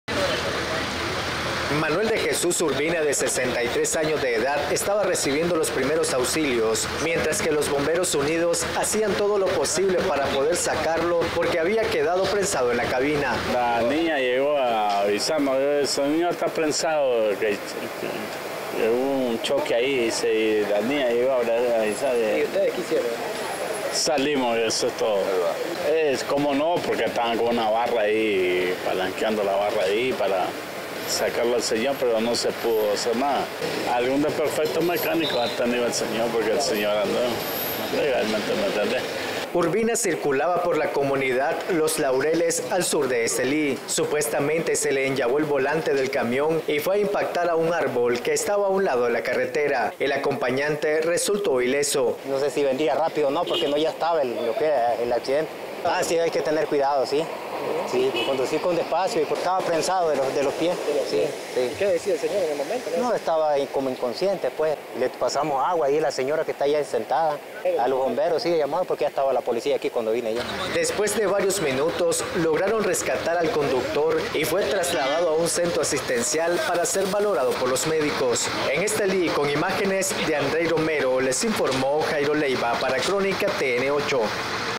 Conductor lesionado tras impactar el camión en un árbol en Estelí
En horas de la tarde del miércoles 30 de agosto del 2023, se registró un accidente de tránsito al sur del municipio de Estelí; donde un conductor perdió el control y se estrelló con el automotor en un árbol.

Manuel de Jesús Alarcón Urbina de 63 años de edad, estaba recibiendo los primeros auxilios; mientras que los bomberos unidos hacían todo posible para poder sacarlo porque había quedado prensando en la cabina.